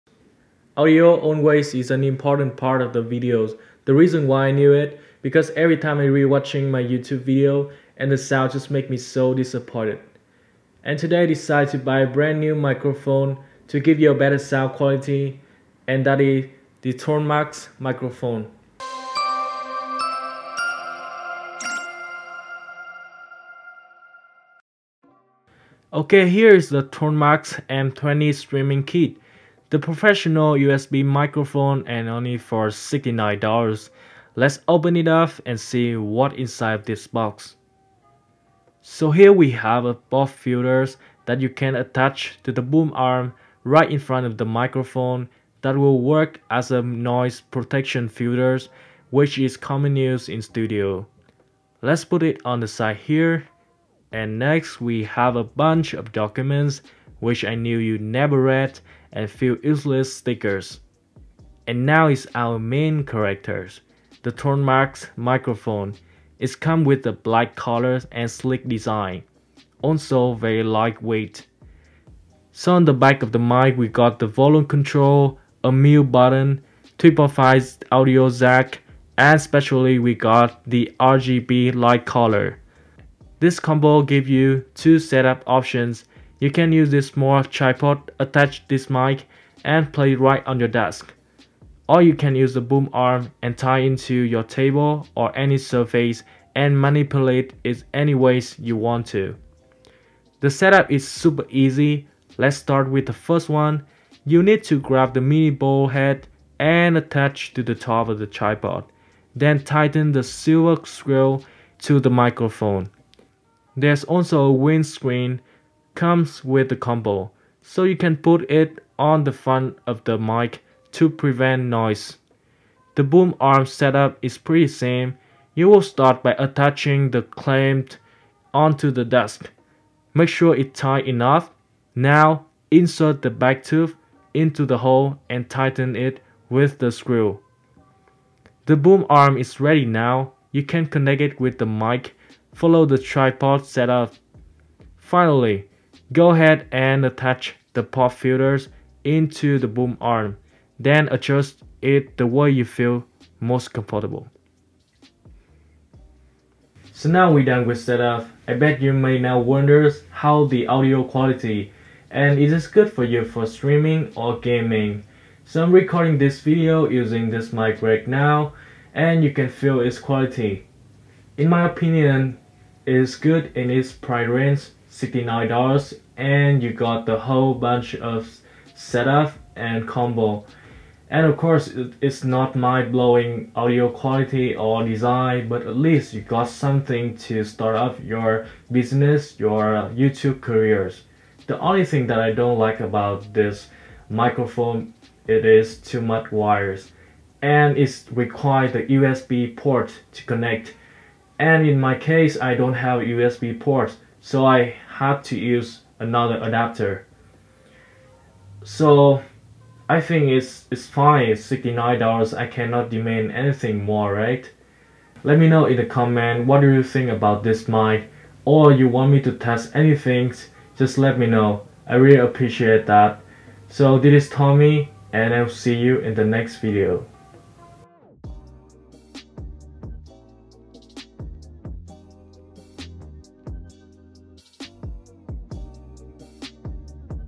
THRONMAX M20 Streaming Kit | Unboxing & Set-up Guide
Recording, live streaming or playing games with the M20 Streaming Kit from Thronmax captures audio at 96 kHz / 24-bit and interfaces with your computer via a shielded, braided USB cable that reduces interference. The M20’s mic offers a USB-C port, and it streams data over USB 2.0.

➣ MacOS Tips & Tricks

MacBook Pro M1
Keychron K1 Keyboard (Version 4)
Samsung Monitor LC24F390FHNXZA
2TB Crucial NVMe SSD Hard Drive